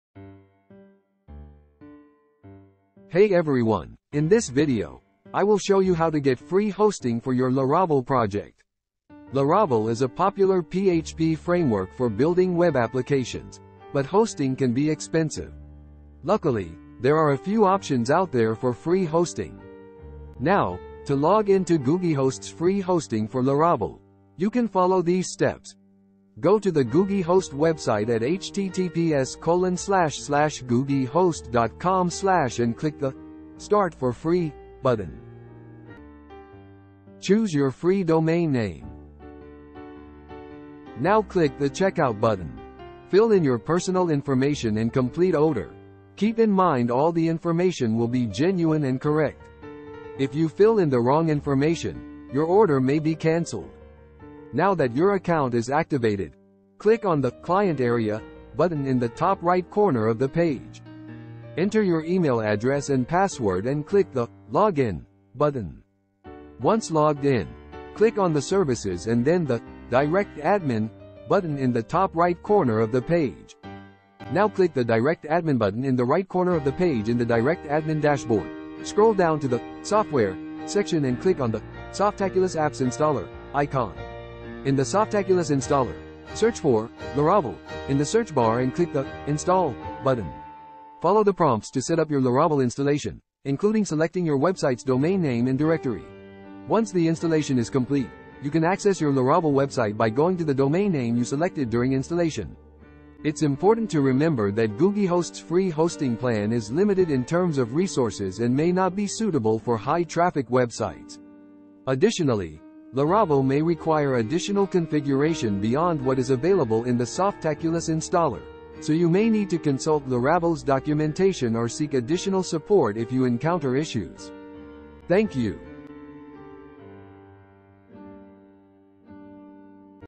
Best Free Laravel Hosting: How to Save Money Without Sacrificing Quality
Build your Laravel site on a budget with our expert recommendations for free hosting providers GoogieHost.com

You can find more on this channel-

Share Video Friends, Family, And World...